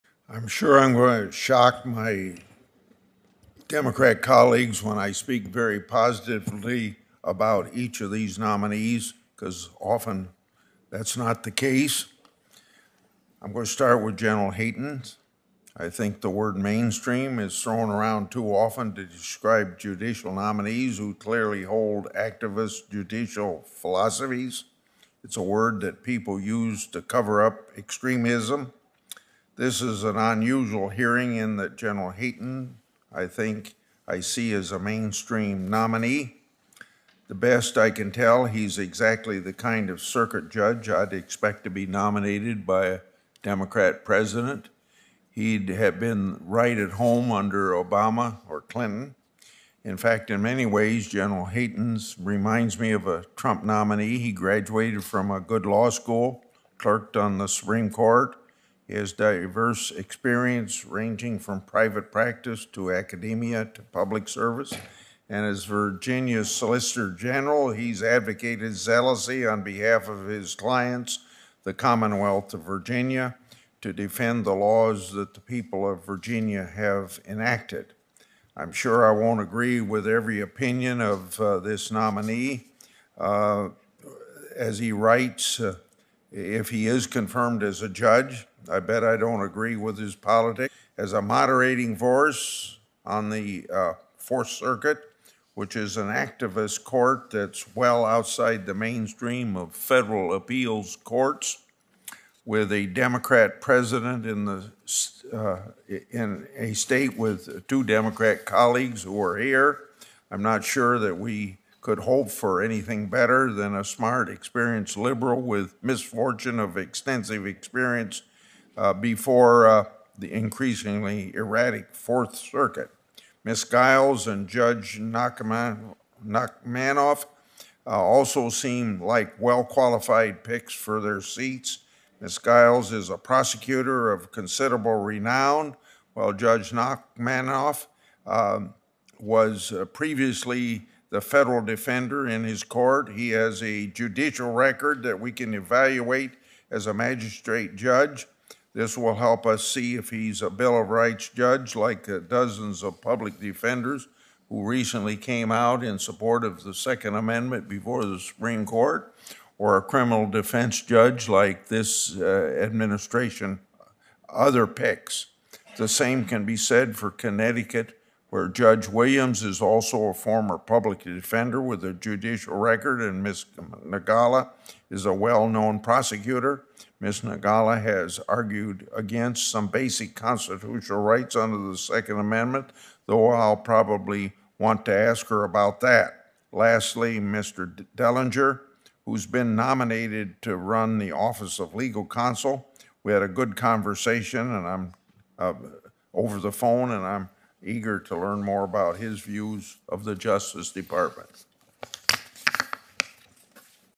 Sen. Grassley: I’ll shock Democrat colleagues by supporting Biden's nominees
In a Senate Judiciary Committee hearing Wednesday, Senator Chuck Grassley said he'll shock his Democrat colleagues by voting in favor of Biden's judicial nominees since it doesn't happen often.